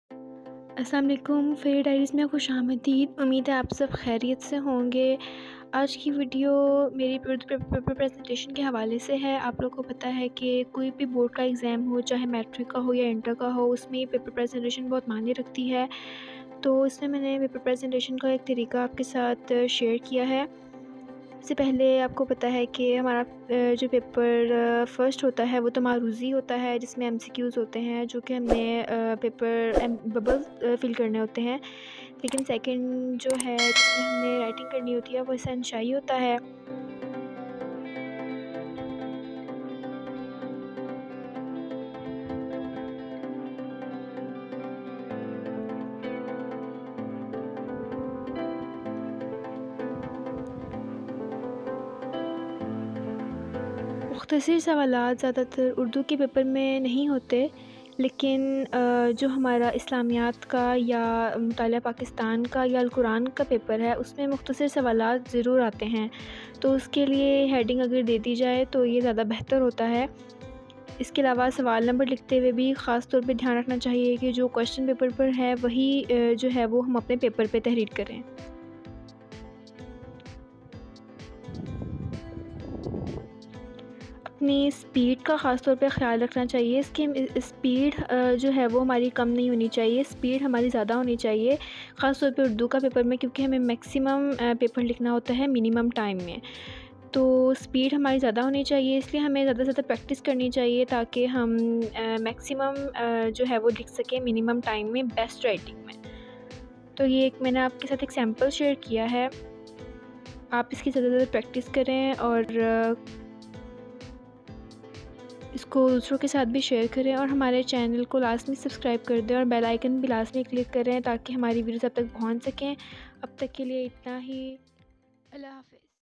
How to improve paper presentation#paperwriting
Hi everyone
this is my channel,, fari diaries,,
this is about art and craft, here is a big change for writing, now you can write in style, so watch this video you will came to know how you can make beautiful your paper presentation,
,, fari diaries,,